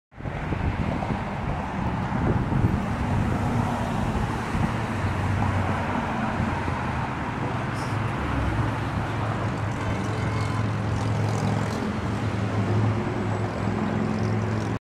Turbo Maserati Ghibli Flyby
Same super saloon, now driving past me. Stately Maserati Ghibli driveway.